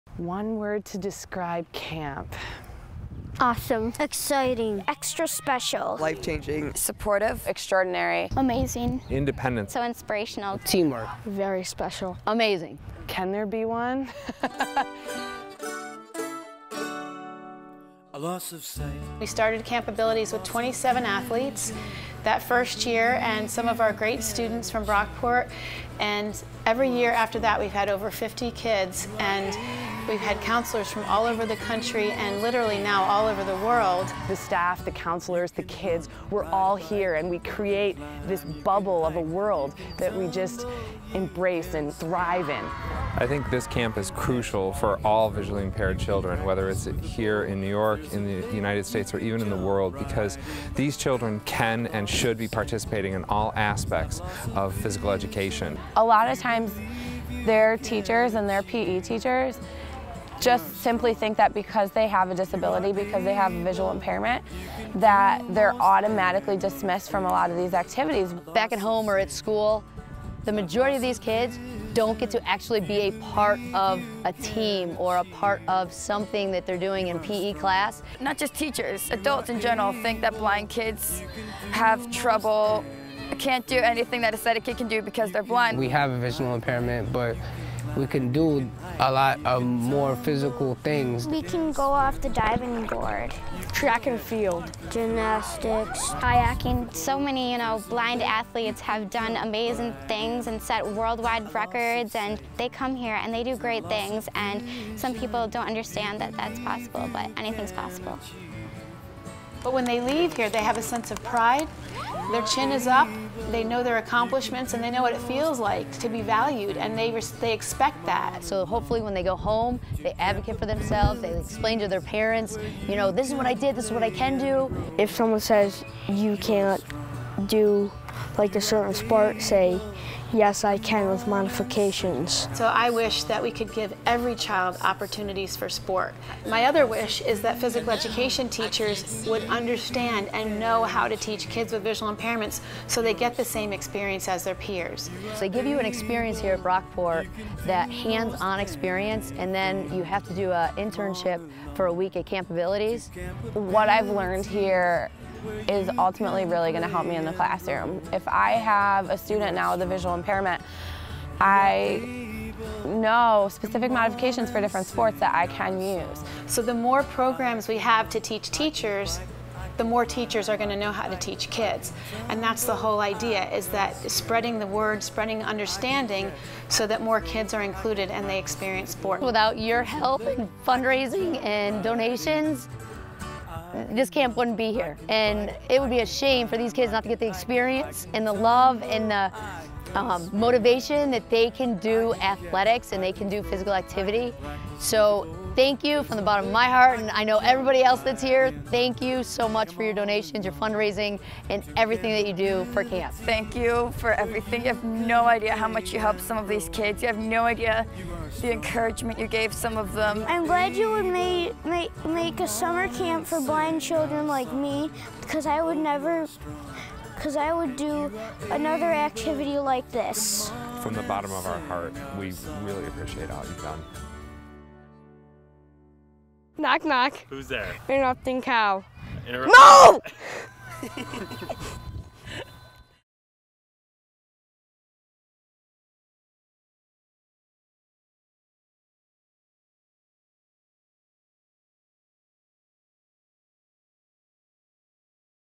What is Camp Abilities About?
Learn about the history and methods of Camp Abilities, a camp for blind and visually impaired students founded and run by Brockport Prof. Lauren Lieberman.
Cinematography by Matt Yeoman and Cathy Craft
Editing by Grace Scamurra